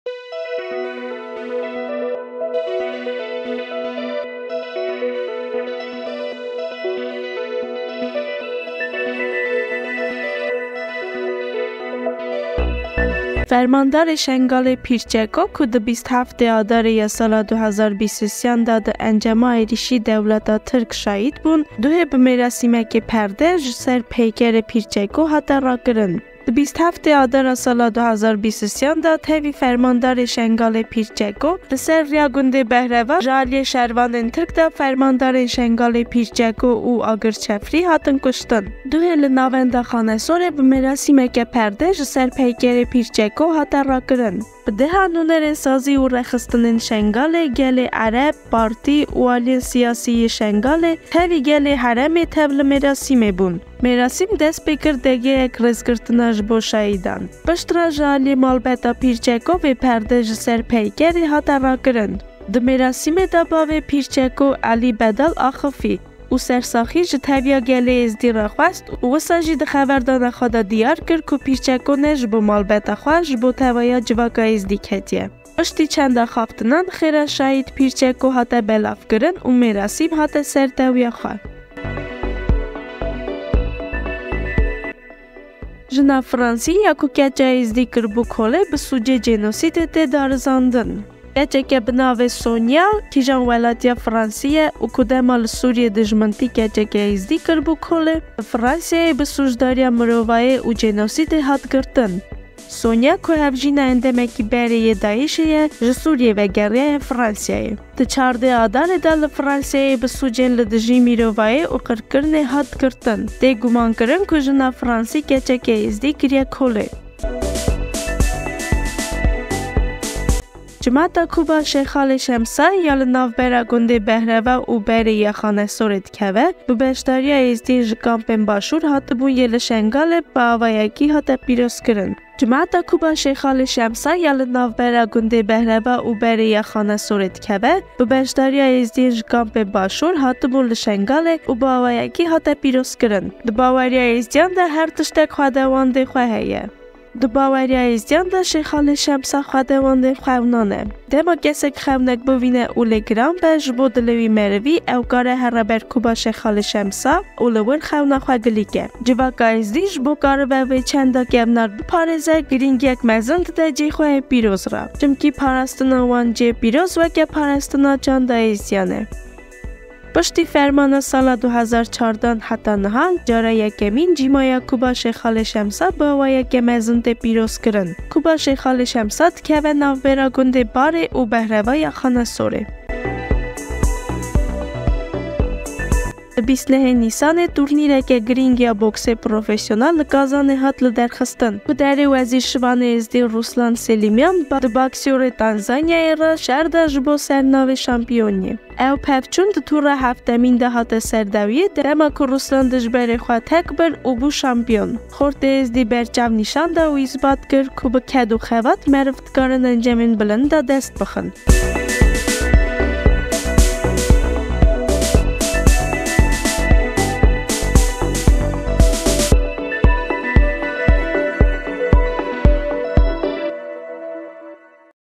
Xîret Nûçe 05.05.2024
1. Di 27’ê Adara 2023’yan de tevi fermanadarê Şengalê Pîr Çeko li ser rêya gundê Behrava ji alyê şervanên tirk de fermandarên Şengalê Pîr Çeko û Agir Cefrî hatin kwiştin.

2. Jina Fransî ya ku keça Êzidî kiribû kole, bi sûcê ‘jenosîdê’ tê darizandin.

3. Cimeya Quba Şêxalê Şemsa ya li navbera gundê Behreva û Barê ya Xanesorê dikeve,  bi beşdariya Êzidiyên ji kampên Başûr hatibûn li Şengalê bi awayeke hate pîrozkirin.

4. Di 29ê Nîsanê da, tûrnîreke girîng ya boksa profesyonal  li Kazanê hate lidarxistin, ku werzişvanê êzîdî Ruslan Selimyan bi baksorê Tanzanyayê ra di şer da ji bo sernavê şampiyonê.

Zêdebin li ser rûpelên me û bişopînin